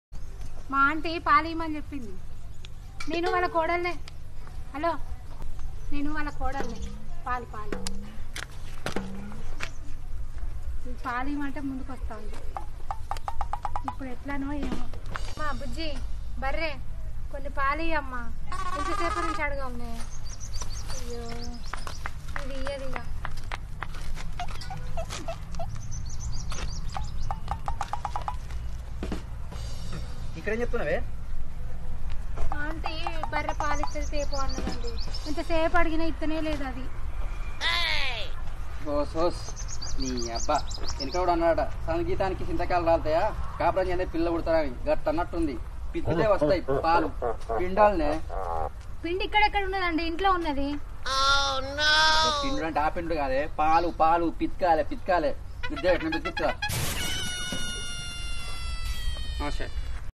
tiktok videos jokes 😂😂😂...... she is asking buffallow to give milk....
she is asking buffaloes to give milk by using her Aunty's name. m